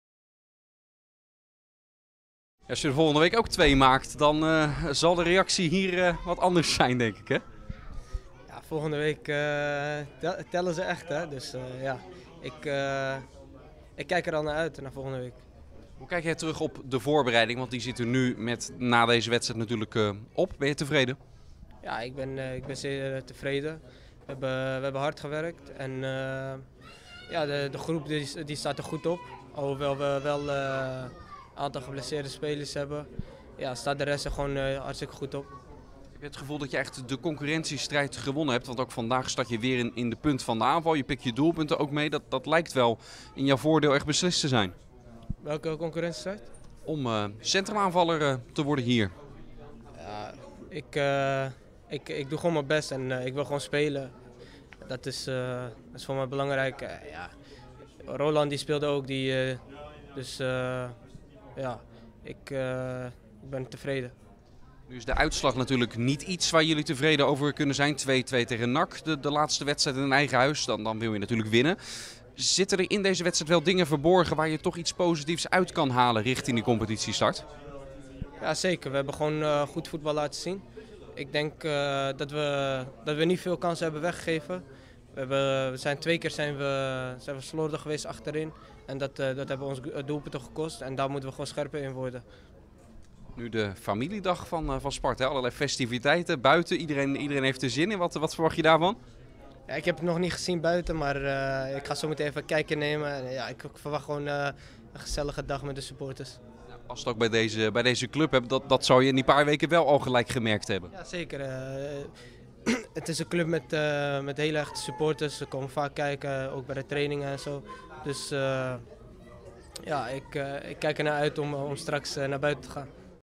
Zakaria El Azzouzi na afloop van Sparta - NAC Breda
Zakaria El Azzouzi was met twee treffers in het oefenduel tegen NAC Breda (2-2) de Spartaan van de wedstrijd. De aanvaller sprak zelf weliswaar van &apos;intikkertjes&apos;, maar hij lijkt zijn draai gevonden te hebben op het Kasteel. &quot;Ik wil Sparta aan handhaving in de eredivisie helpen&quot;, aldus El Azzouzi tijdens de jaarlijkse Familiedag.De door Ajax aan Sparta verhuurde speler wil vooral veel minuten maken in de eredivisie en is blij dat hij goed is opgevangen als nieuweling. Met name over het kampioensfeest van afgelopen seizoen hoort hij regelmatig wat. &quot;Ik heb veel filmpjes gezien. De jongens hebben het er nog steeds over. Het was een gekkenhuis.&quot;Zijn teamgenoot Paco van Moorsel maakte dat feest mee. &quot;Het kampioenschap was speciaal. Het feest was een mooie afsluiter van een mooi seizoen.&quot; Het nieuwe seizoen, dat volgende week begint, wordt vanzelfsprekend een stuk lastiger. &quot;Het verschil zit hem dit seizoen in kwaliteit. Daar moeten we op anticiperen. Dat hebben we al gedaan met het halen van nieuwe jongens, maar we moeten ook anders voetballen dan in de Jupiler League&quot;, aldus Van Moorsel.